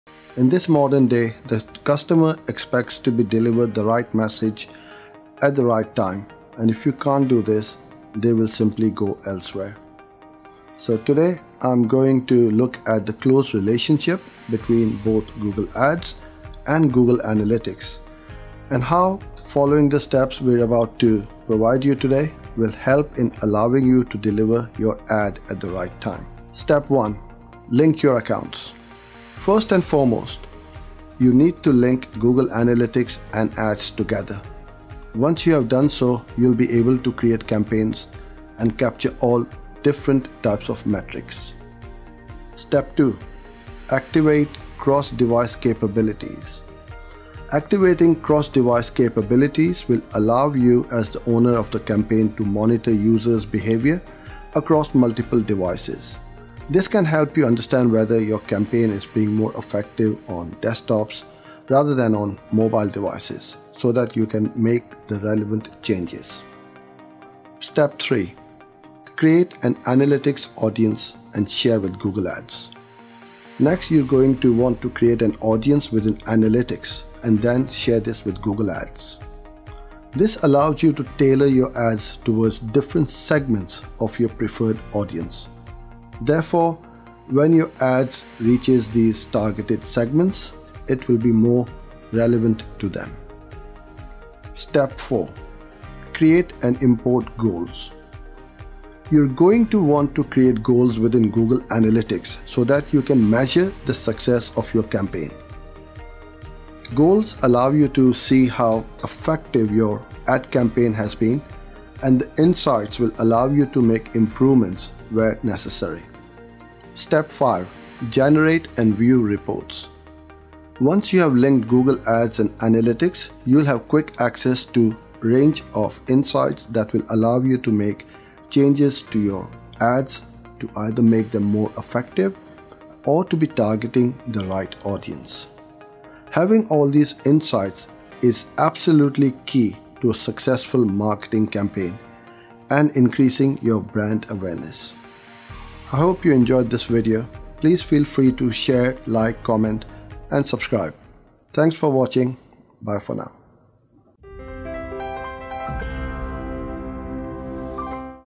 Google Ads Tutorial 2020 | How to Improve Google Ads Performance Using Google Analytics
Plus join the Livestream every weekday at 1600 hrs UK time

Want to improve your Google Ads performance? Here’s why you should link Google Analytics to Google Ads. 5 top tips for you to get started.

Free Resources